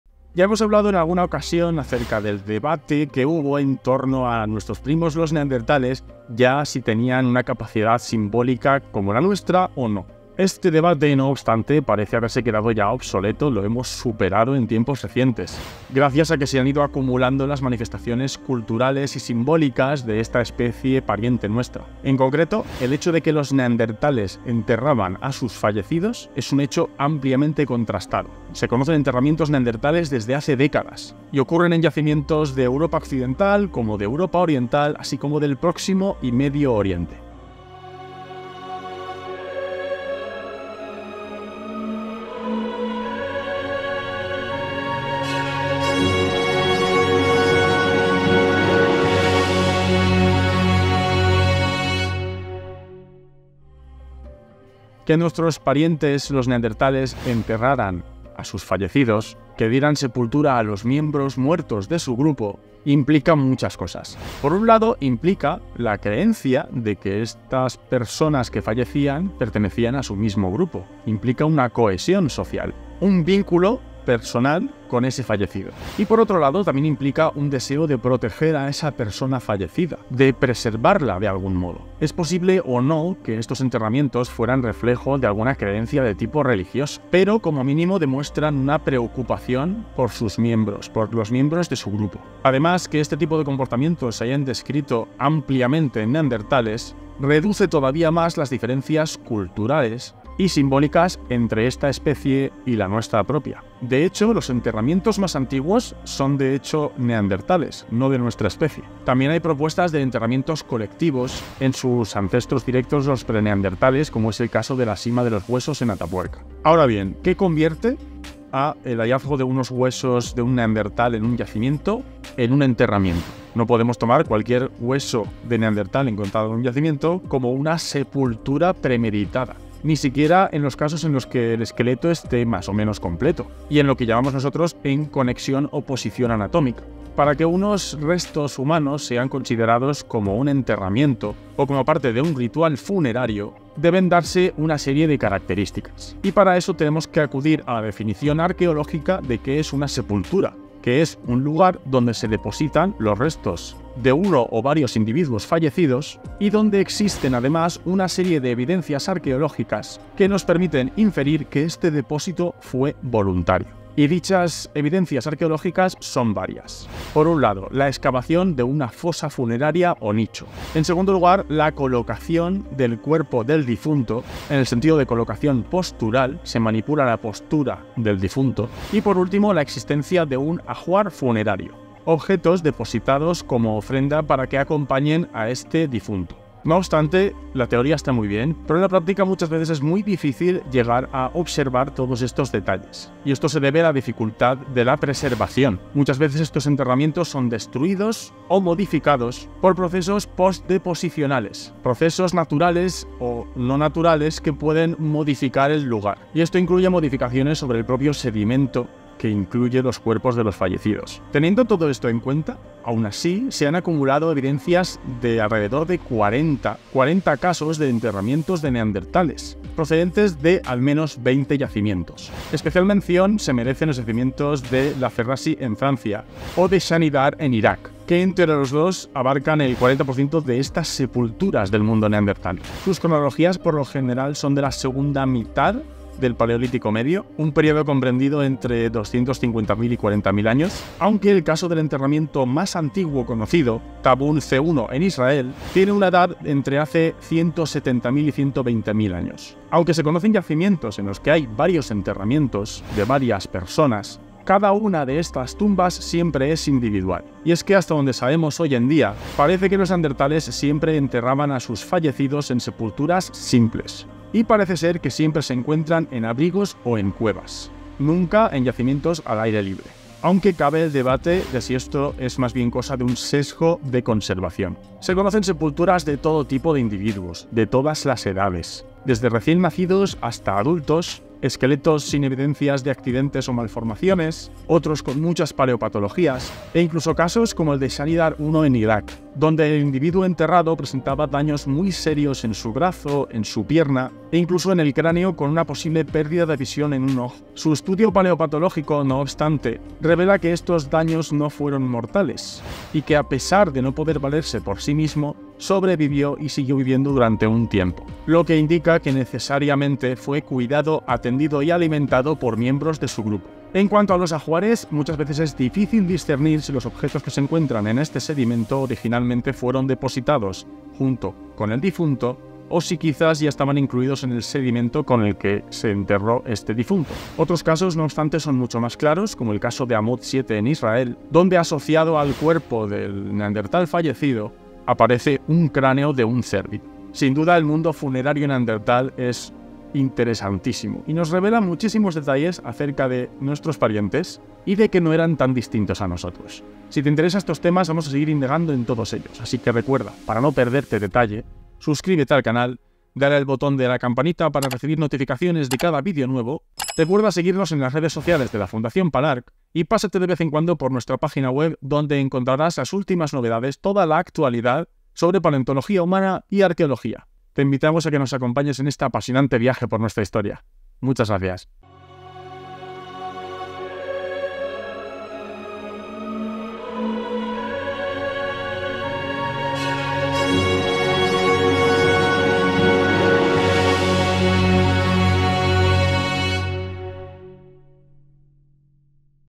Más allá de los huesos: El emotivo misterio de los enterramientos neandertales
En este fascinante vídeo, exploramos el sorprendente comportamiento simbólico de los neandertales, resolviendo un antiguo debate sobre si tenían prácticas funerarias. Descubriremos cómo los neandertales enterraban a sus muertos, lo que implica una profunda conexión con los fallecidos y una preocupación por proteger y preservar a sus seres queridos. Analizaremos las características que hacen que un hallazgo sea considerado como un enterramiento, destacando las evidencias arqueológicas clave, como la colocación postural del cuerpo y la presencia de ajuares funerarios.

Exploraremos los increíbles yacimientos de La Ferrassie en Francia y Shanidar en Irak, que albergan una gran parte de las sepulturas conocidas del mundo neandertal. Viajaremos a través del tiempo, desde el paleolítico medio hasta aproximadamente 120,000 años atrás, para descubrir el enterramiento más antiguo conocido hasta la fecha en Tabun C1, Israel.

Además, analizaremos la diversidad de los enterramientos neandertales, desde individuos de todas las edades hasta aquellos con paleopatologías e incapacidades físicas, lo que nos revela la cuidadosa atención y apoyo que brindaban a sus miembros más vulnerables. También exploraremos los enigmas en torno a los ajuares funerarios y cómo nos ayudan a comprender la rica cultura de nuestros antepasados.

Únete a nosotros en este emocionante viaje a través de la arqueología y la paleontología mientras desentrañamos los misterios de los antiguos rituales funerarios de los neandertales y cómo estos descubrimientos nos conectan más profundamente con nuestra historia ancestral.